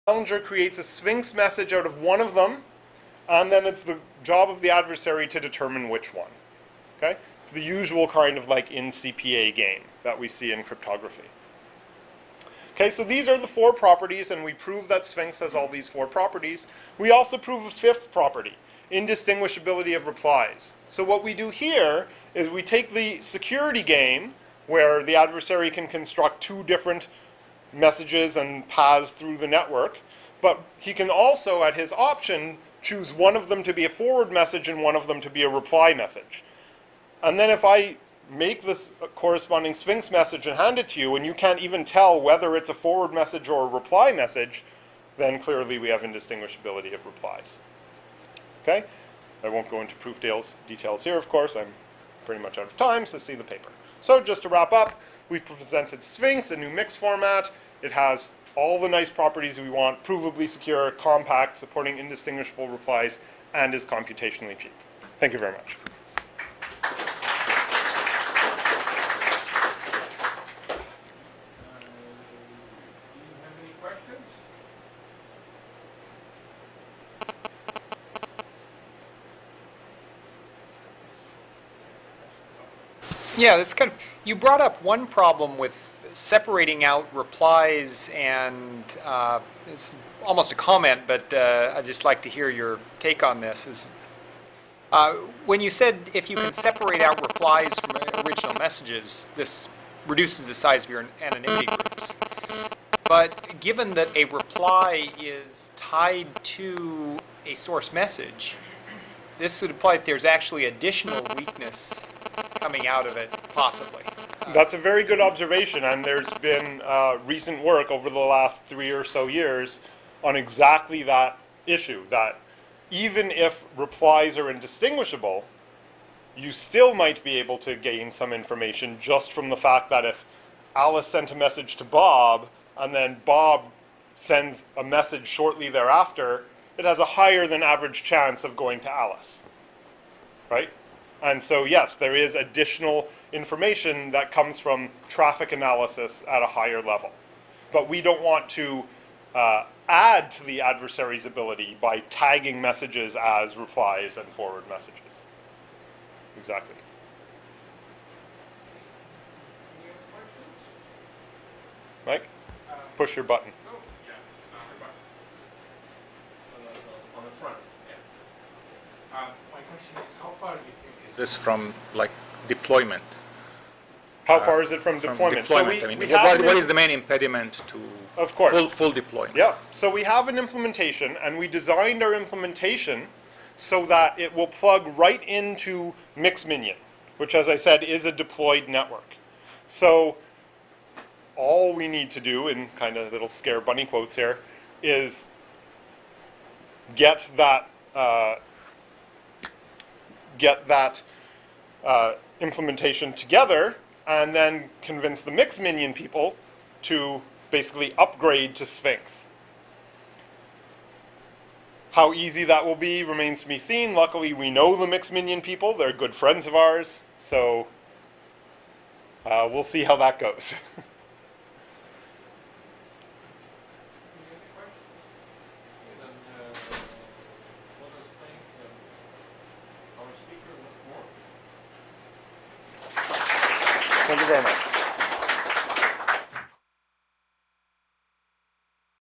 CERIAS Security: Sphinx: A Compact and Provably Secure Mix Format 6/6
Clip 6/6
Speaker: Ian Goldberg · University of Waterloo

Mix networks, originally proposed in 1981, provide a way for Internet
users to send messages--such as email, blog posts, or tweets--without
automatically revealing their identities or their locations. In this
talk, we will describe Sphinx, a cryptographic message format used to
relay anonymized messages within a mix network. It is the first scheme
to support a full set of security features: compactness, efficiency,
provable security, indistinguishable replies, hiding the path length and
relay position, as well as providing unlinkability for each leg of the
message's journey over the network. We will compare Sphinx to other mix
formats, and will also briefly outline Sphinx's security reduction
proof.